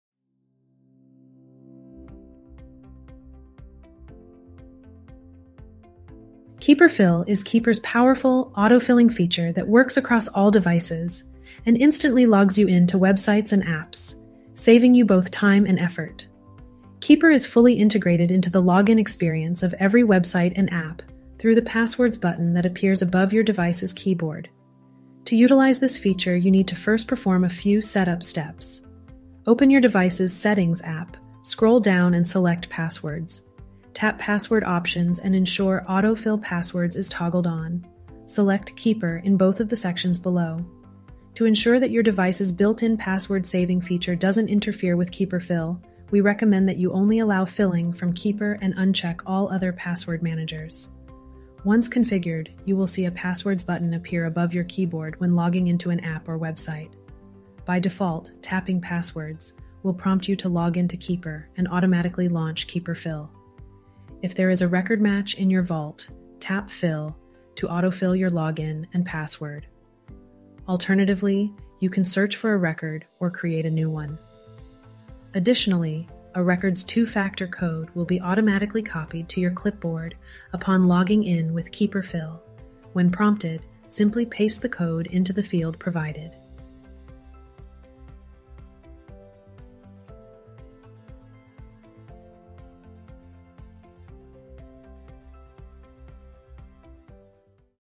Keeper 101 - How to Set Up and Use KeeperFill on iOS
KeeperFill is Keeper’s powerful, autofilling feature that works across all devices and instantly logs you in to websites and apps, saving you both time and effort.

Keeper is fully integrated into the login experience of every website and app through the Passwords button that appears above your device's keyboard. To utilize this feature you need to first perform a few setup steps.

Open your device's “Settings” app, scroll down and select Passwords. Tap Password Options and ensure "AutoFill Passwords" is toggled on. Select “Keeper” in both of the sections below. To ensure that your device's built-in password saving feature doesn't interfere with KeeperFill, we recommend that you only allow filling from Keeper and uncheck all other password managers.

Once configured, you will see a Passwords button appear above your keyboard when logging into an app or website. By default, tapping Passwords, will prompt you to log in to Keeper and automatically launch KeeperFill. If there is a record match in your vault, tap Fill to autofill your login and password.

Alternatively, you can search for a record or create a new one. Additionally, a record’s two-factor code will be automatically copied to your clipboard upon logging in with KeeperFill. When prompted, simply paste the code into the field provided.